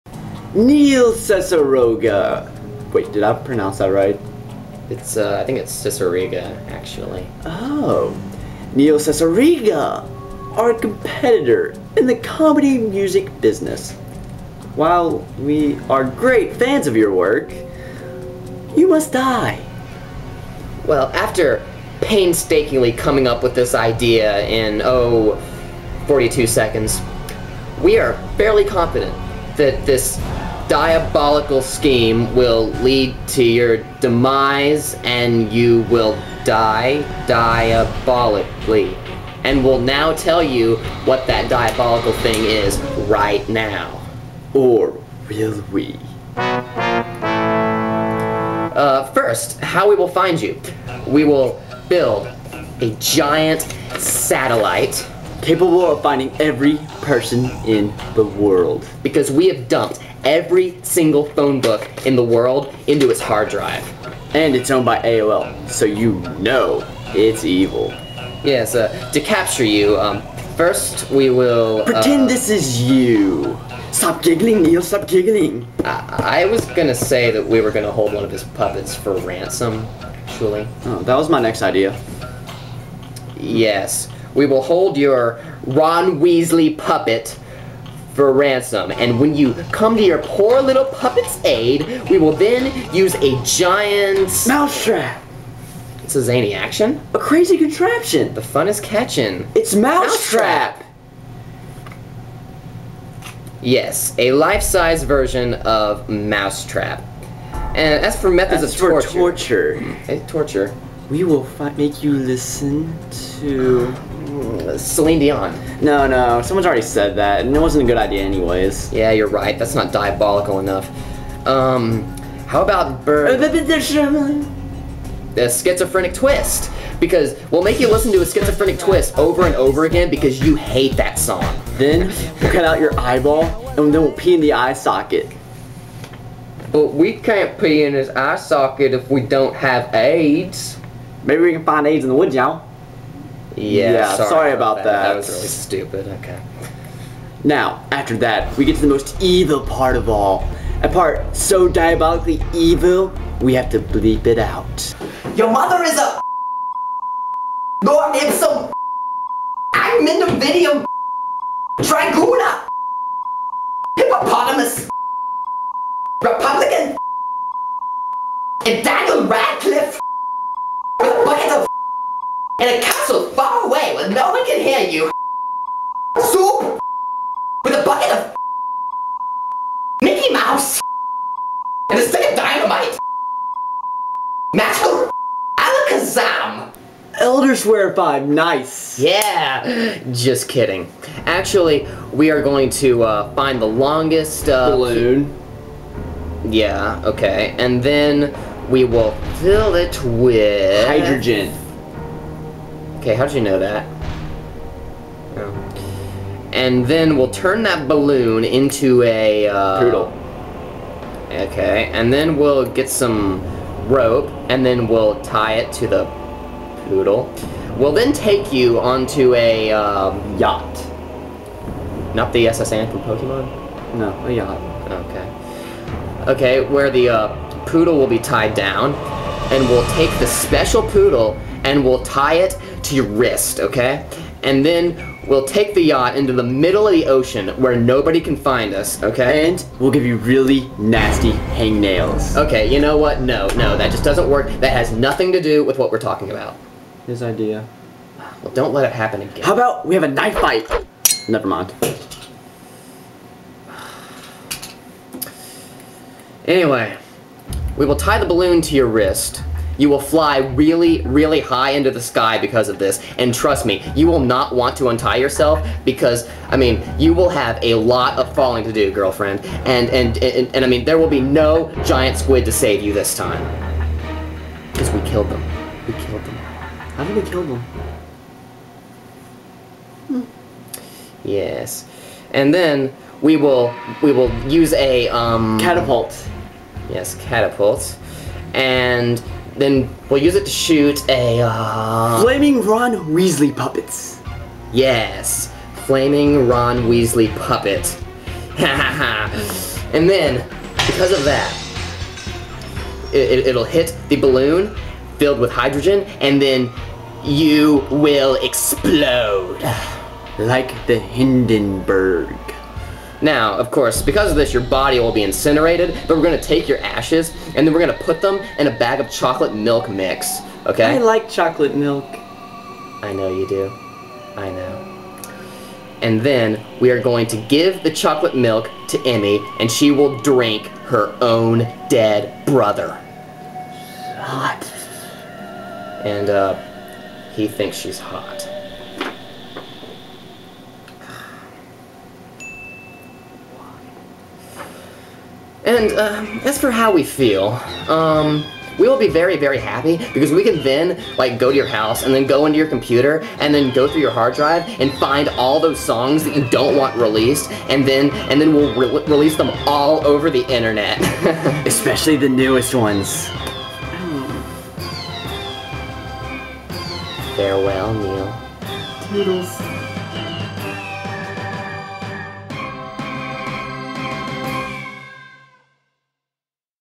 Re: Send Neil death threats
Jace McLain and Evan Topp bring you... the ultimate death threat. Or something.